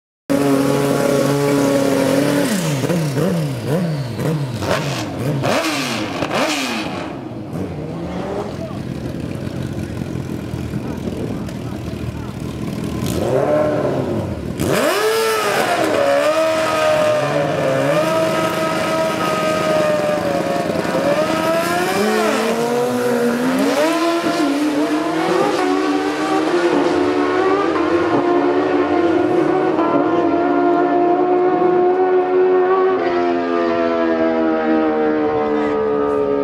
November 2012 RWUB Run 73
10th November 2012 - The last Run What You Bring Event for 2012, Held @ Hal Far Raceway!!